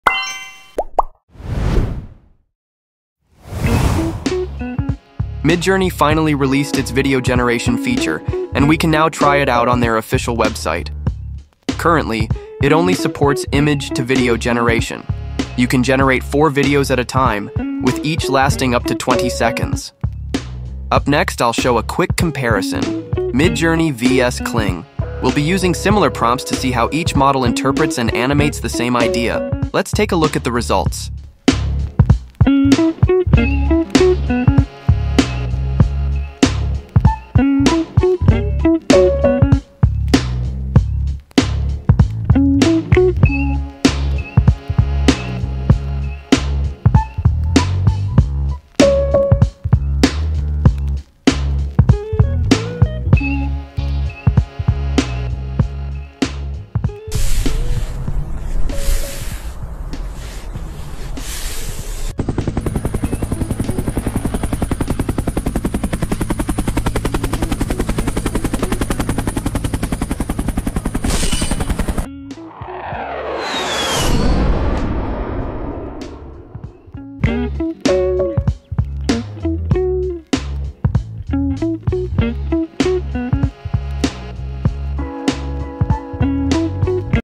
Midjourney V1 Video Model VS Kling AI | Which One Wins?
Midjourney just released V1 Video Model, in this video, I compare Midjourney with Kling AI using similar prompts. Which is better for you?

Chapters:
00:00 Brief Intro.
00:30 Midjourney VS Kling AI